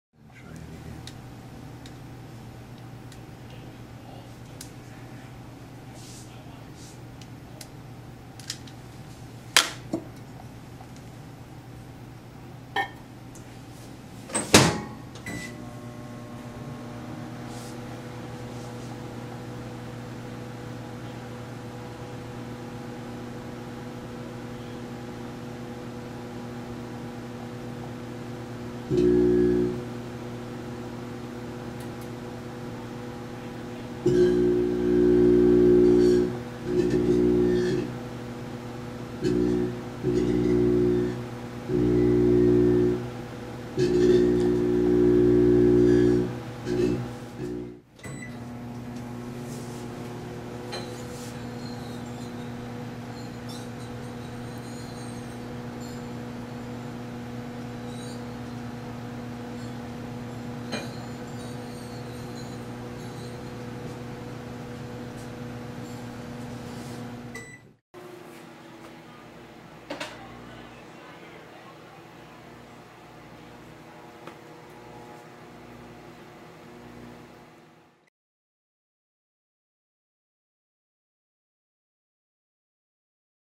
microwave_experiments
Three short clips of a microwave oven.  The first clip show creation of a microwave plasma blob.  The second clip shows the plasma blobs forming in lightbulbs, including a neon bulb, as they rotate through the microwave fields.  When going through microwave nodes, the plasma goes out.  The third clip shows hundreds of neon tubes mounted in styrofoam rotating through the microwave fields, revealing the changing pattern of nodes and antinodes.